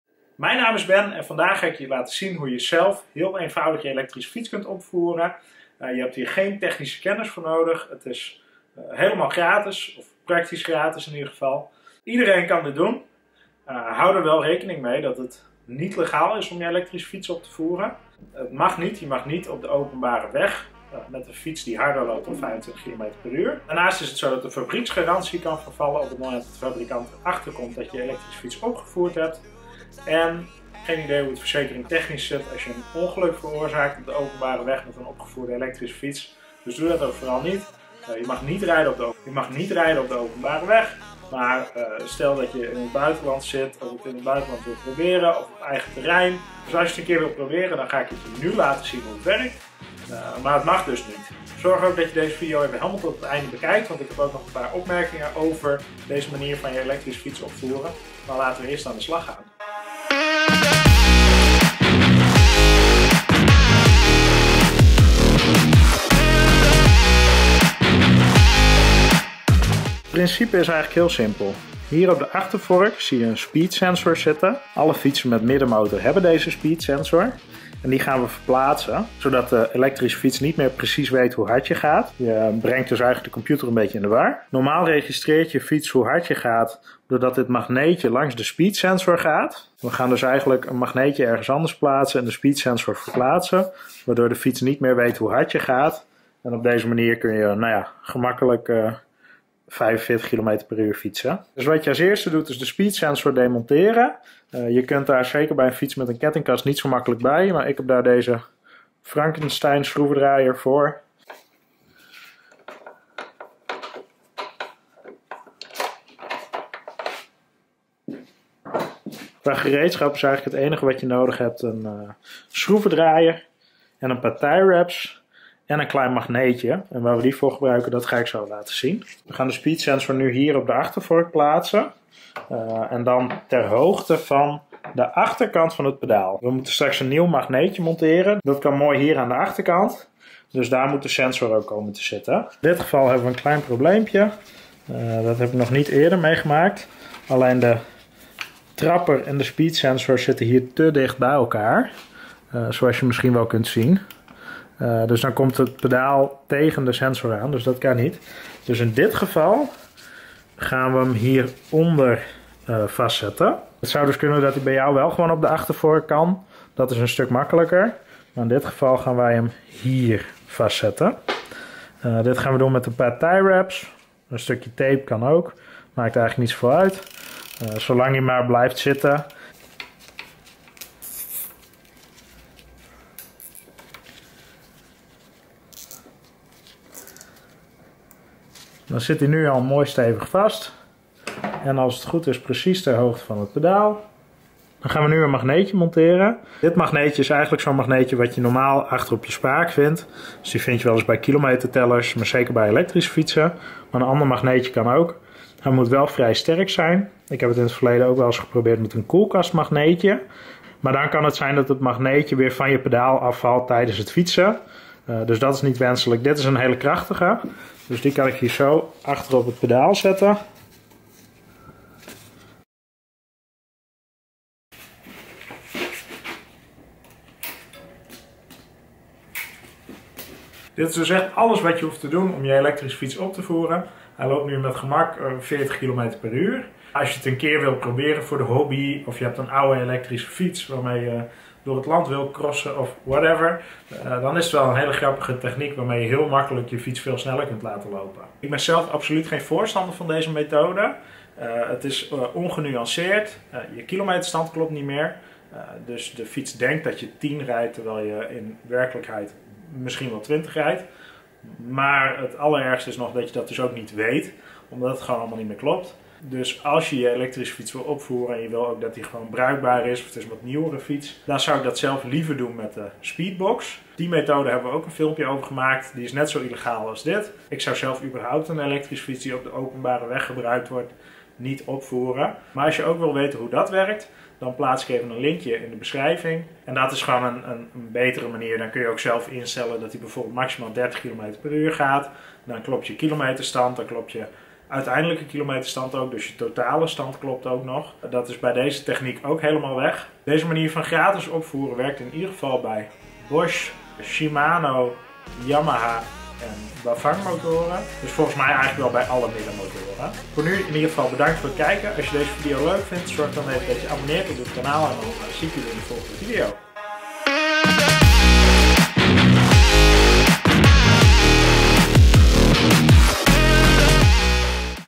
Gratis E-Bike opvoeren - Elektrische fiets op voeren met simpel trucje
Wil je nog even teruglezen hoe dit werkt?

Of wil je liever je fiets opvoeren door middel van een speedbox? Bekijk dan deze video even

Meer weten over E-Bike tuning?

Wil jij je E-Bike opvoeren maar liever geen geld uitgeven? Met dit trucje kun je je fiets harder laten lopen. Let op: Dit is illegaal en niet toegestaan op de openbare weg. Op deze manier kan iedereen zelf zijn elektrische fiets opvoeren in een paar minuten. Houd er wel rekening mee dat dit alleen voor gebruik op eigen terrein is.